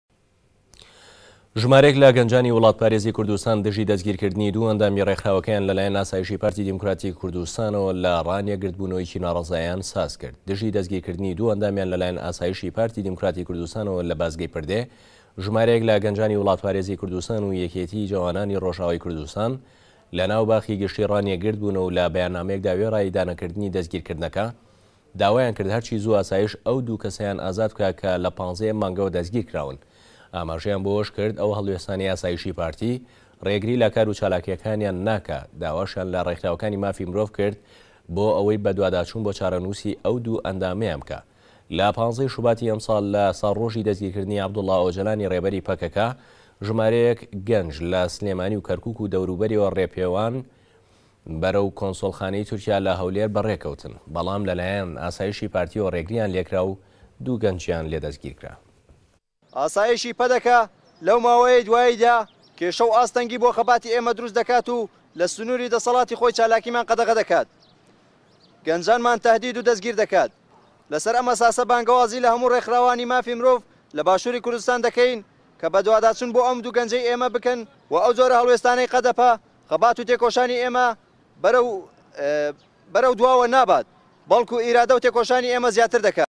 گه‌نجانی وڵاتپارێز: ئاسایشی پارتی دوو ئه‌ندامى گرتووین
ژماره‌یه‌ك له‌گه‌نجانی وڵاتپارێزی كوردستان دژی ده‌ستگیر كردنی دوو ئه‌ندامی ڕێكخراوه‌كه‌یان له‌لایه‌ن ئاسایشی پارتی دیموكراتی كوردستانه‌وه‌ له‌ ڕانیه‌ گردبونه‌وه‌یه‌كی ناڕه‌زاییان سازكرد.

دژی ده‌ستگیر كردنی دوو ئه‌ندامیان له‌لایه‌ن ئاسایشی پارتی دیموكراتی كوردستانه‌وه‌ له‌ بازگه‌ی پردێ، ژماره‌یه‌ك له‌ گه‌نجانی وڵاتپارێزی كوردستان و یه‌كێتی جه‌وانانی ڕۆژئاوای كوردستان، له‌ناوباخی گشتی ڕانیه‌، گردبونه‌وه‌ و له‌ به‌یاننامه‌یه‌كدا وێڕای ئیدانه‌كردنی ده‌ستگیر كردنه‌كه‌ داوایانكرد هه‌رچی زووه‌ ئاسایش ئه‌و دوو كه‌سه‌یان ئازاد بكات كه‌ له‌15ی ئه‌م مانگه‌وه‌ ده‌ستگیر كراون.

ئاماژه‌یان بۆ ئه‌وه‌شكرد؛ ئه‌و هه‌ڵوێستانه‌ی ئاسایشی پارتی، ڕێگری له‌ كار و چالاكییه‌كانیان ناكات، داواشیان له‌ ڕێكخراوه‌كانی مافی مرۆڤ كرد بۆ ئه‌وه‌ی به‌دواداچوون بۆ چاره‌نوسی ئه‌و دوو ئه‌ندامه‌یان بكات.

له‌ 15ی شوباتی ئه‌مساڵ له‌ ساڵڕۆژی ده‌ستگیر كردنی عه‌بدولاَ ئۆجه‌لانی ڕێبه‌ری په‌كه‌كه‌، ژماره‌یه‌ك گه‌نج له‌ سلێمانی و كه‌ركوك و ده‌وروبه‌رییه‌وه‌ به‌ڕێپێوان به‌ره‌و كونسڵخانه‌ی توركیا له‌ هه‌ولێر به‌ڕێكه‌وتن، به‌ڵام له‌لایه‌ن ئاسایشی پارتییه‌وه‌ ڕێگریان لێكرا و دوو گه‌نجیان لێ ده‌ستگیركرا.